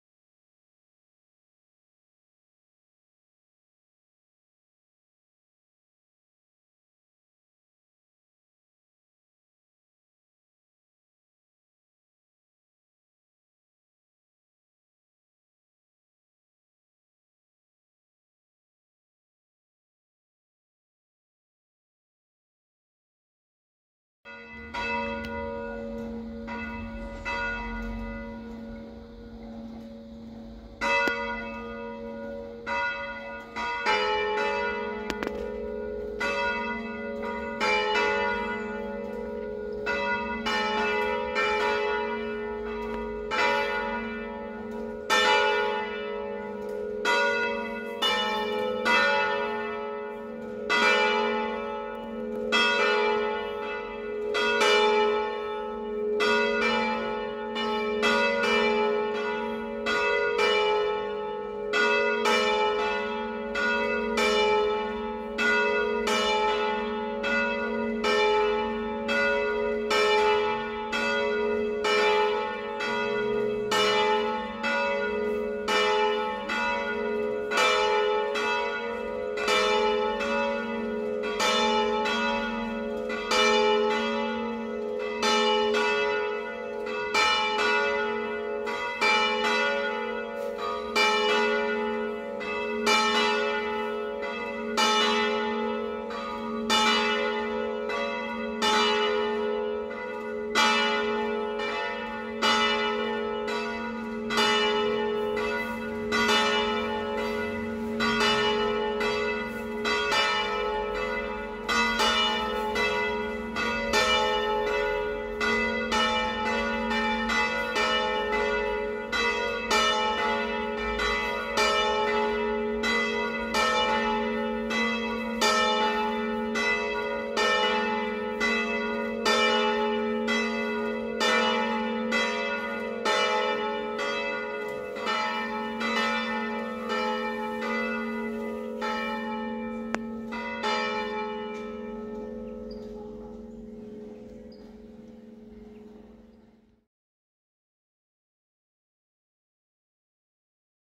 Le Campane di Rio Marina (LI) - Suonata Festiva
📍 Rio Marina (LI)

⛪ Chiesa Parrocchiale di Santa Barbara Vergine e Martire

🔔 Concerto di 4 Campane in La3*
      Magni (?)

Suonata Festiva con La3 e Si3 in occasione della Santa Messa.

*Viste da sotto, pare che su 4 si possano muovere solo le due maggiori, mentre le due più piccole sono fisse. Se qualcuno avesse qualche info in più su questo concerto, scriva pure nei commenti